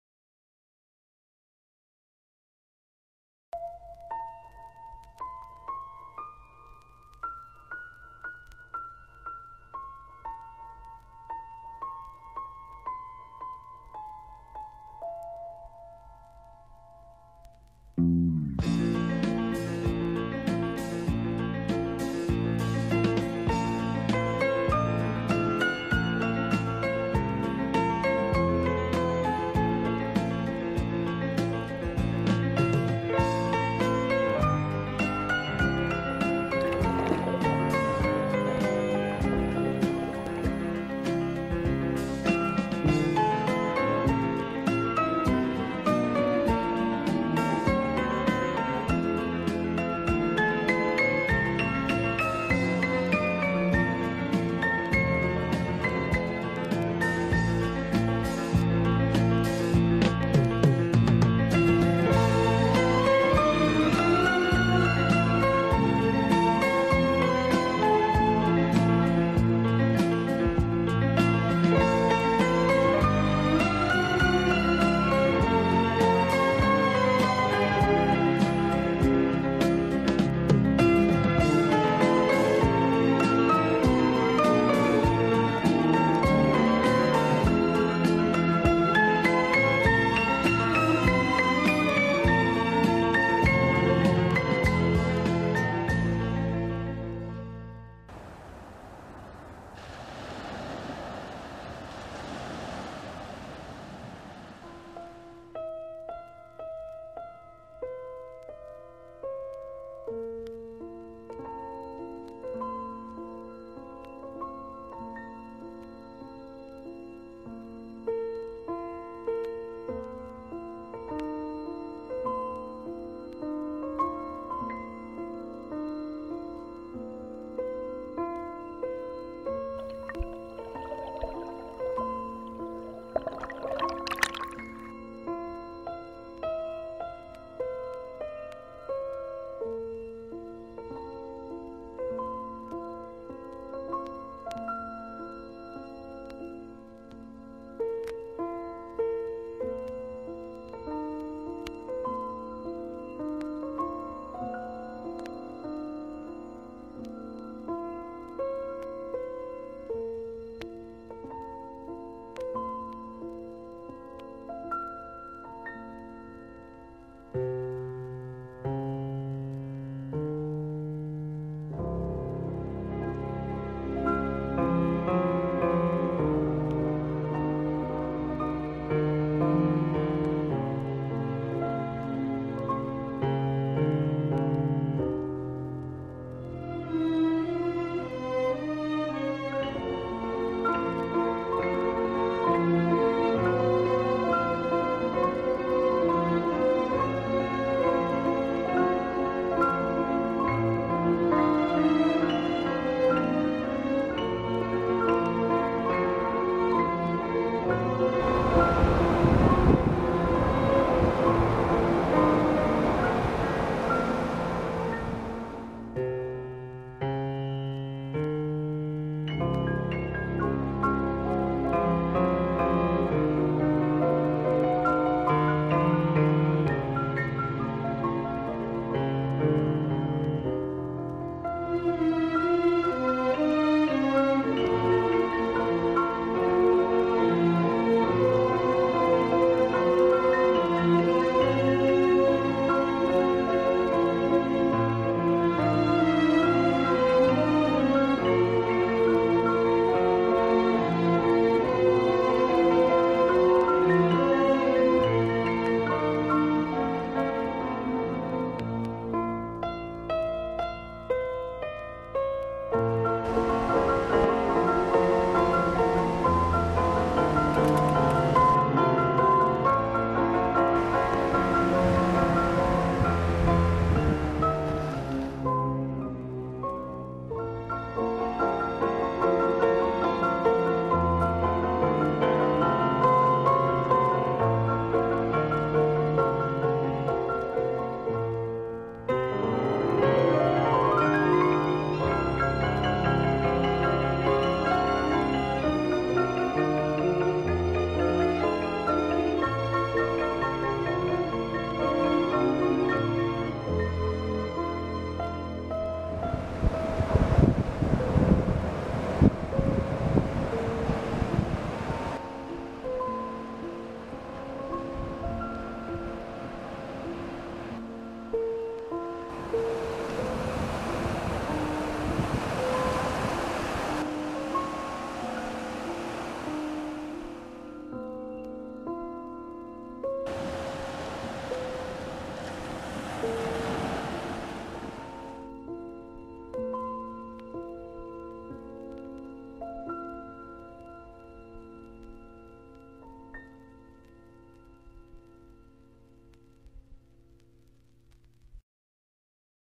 Maine 2015, York, Cape Neddick
Encore une fois dans le Maine, a York, avec notre Columbus, superbe mois de septembre on voit et revoit les mêmes places pour la "n"-eme fois sans se lasser et comme Paulo Cuelho le disait : " fait attention a l'endroit ou tu va pleurer parce que c'est la que ce trouve ton trésor et c'est la que ce trouve ton coeur.